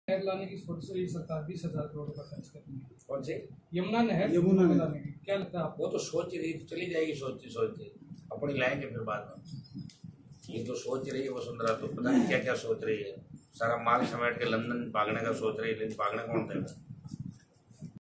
वसुंधरा सारा माल समेट कर लंदन भागने की सोच रही हैं लेकिन भागने कौन देगा-हनुमान बेनीवाल
जयपुर। किसान हुंकार महारैली कर अपनी आवाज बुलंद करने वाले खींवसर विधायक हनुमान बेनीवाल बीजेपी और कांग्रेस दोनों पर जमकर बरसे। सीएम राजे पर तीखा हमला करते हुए बेनीवाल ने कहा कि वसुंधरा सारा माल समेटकर भागने की तैयारी में हैं।
10 जून को सीकर में आयोजित हुई किसान हुंकार रैली से सरकार तक अपनी बात पहुंचाने वाले विधायक हनुमान बेनीवाल ने दोनों पार्टियों को आड़े हाथों लिया। उन्होंने कहा कि इस रैली की गूंज दिल्ली तक जाएगी। इस मौके पर बेनीवाल ने भाजपा और कांग्रेस को आड़े हाथों लेते हुए कहा कि दोनों पार्टी बारी-बारी सत्ता में आसीन होकर राजस्थान की जनता के साथ धोखा कर रहे हैं।
बेनीवाल ने आरोप लगाते हुए कहा कि अब तक रही सरकारों ने किसानों गरीबों और बेरोजगारों की कोई सुनवाई नहीं की। उन्होंने कहा कि इस रैली की गूंज दिल्ली तक जाएगी जिससे भाजपा और कांग्रेस दोनों का दिमाग ठीक होगा। वहीं रैली से पहले की गई पत्रकार वार्ता में उन्होंने सीएम राजे पर कई तीखे हमले किए।
यमुना नहर के सवाल पर बेनीवाल ने कहा कि वो सिर्फ सोचती हैं और सोचते सोचते ही सरकार चली जाएगी। उन्होंने कहा कि वसुंधरा सरकार सारा माल समेटकर लंदन भागने की तैयारी में है।